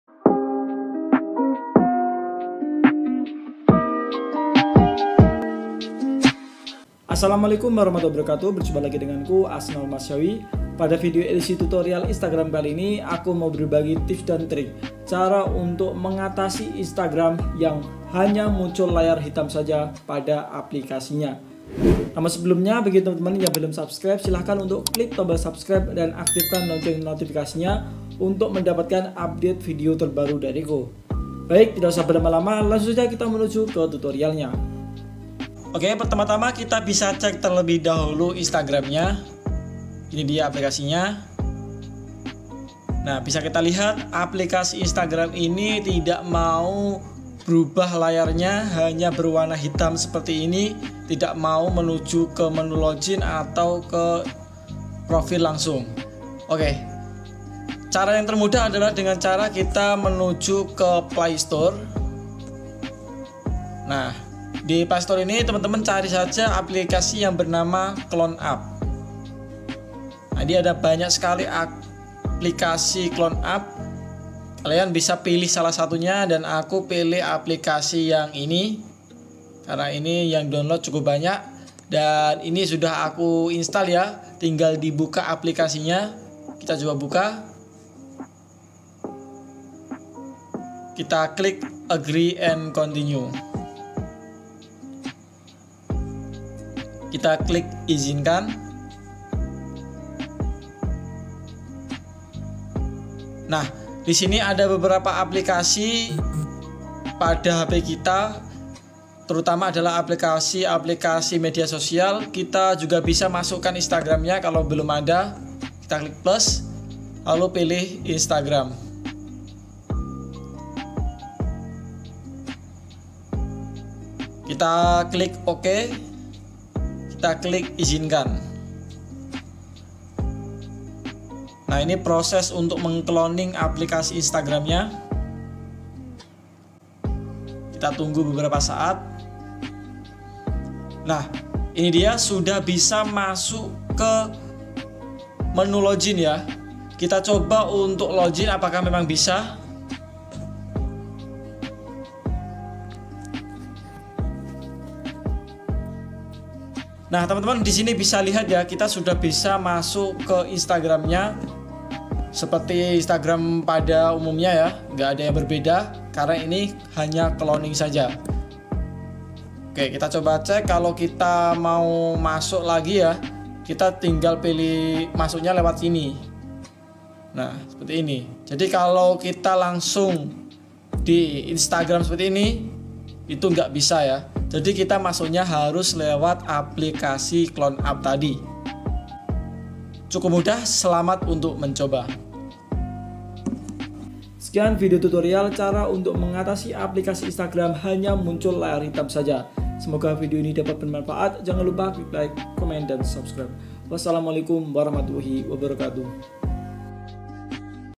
Cara Mengatasi Instagram Layar Hitam
Video Edisi tutorial kali ini yaitu tentang cara mengatasi aplikasi instagram hanya muncul warna hitam atau blank tidak bisa dibuka dengan sangat mudah.

Selamat mencoba!